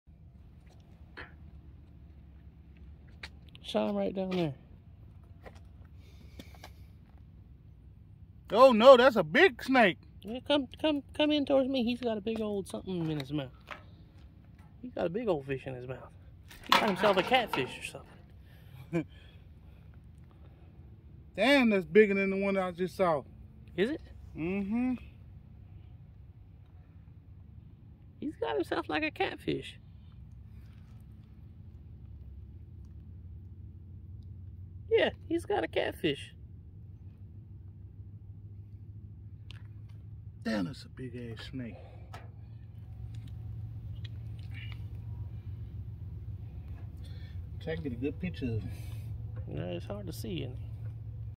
catfish caught by snake
The brown water snake (Nerodia taxispilota) is one of the most common water snakes in North America and is found mainly in swamps and streams in lower coastal regions in North Carolina¹²⁴. They are almost completely brown, with the exception of around 25 black or dark brown markings down their back. They have thick, heavy bodies and large, flat heads². Brown water snakes are primarily found in rivers or large streams in the Coastal Plain of North Carolina; however, they do extend into Piedmont along large river systems³.